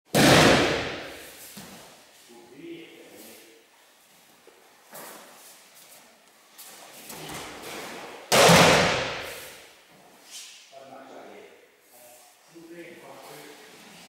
Aikidoka Kurt and AIKIDO videos...
Aikidoka Kurt From Belgium and other Aikidokas train on stage with SHIHAN Jacques Horny from Belgium 6° DAN AIKIDO Aikikai Rokudan in dojo Rebecq Belgium on Saturday, June 14, 2014 ... :-)